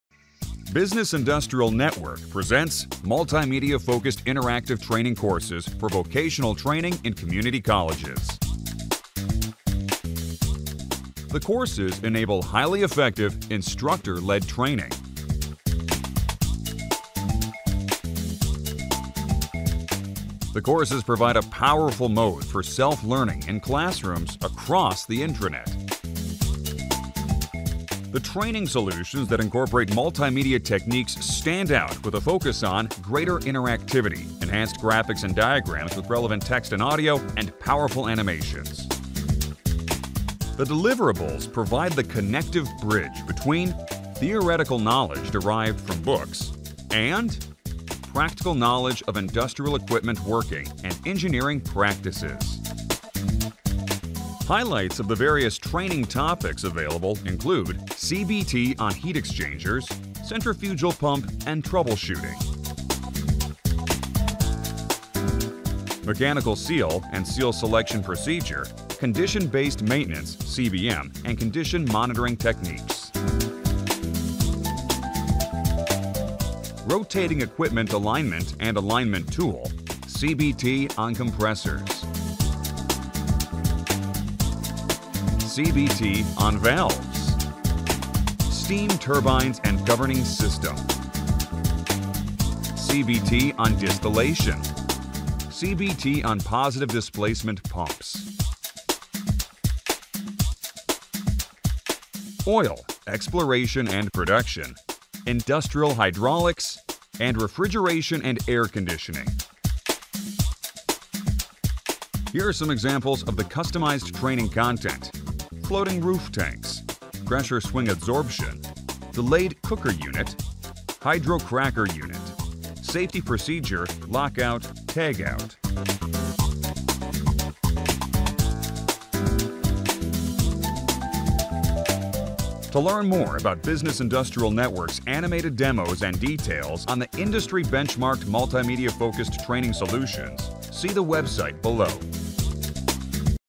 Mechanical Engineering Classes
Introduction to Mechanical Engineering and Maintenance training software site licenses.

These certificate courses are great for technical schools, vocational schools and company training department to compliment current mechanical engineering classes, curriculum and books with practical training for students and employees.
Courses and topics include but are not limited to Air Compressor, HVAC, Centrifugal Pumps and Troubleshooting, Heat Exchangers, Mechanical Seals, Condition Based Maintenance, Rotating Equipment Alignment, Valves, Steam Turbines, Distillation, Oil Exploration and Production, Industrial Hydraulics, etc.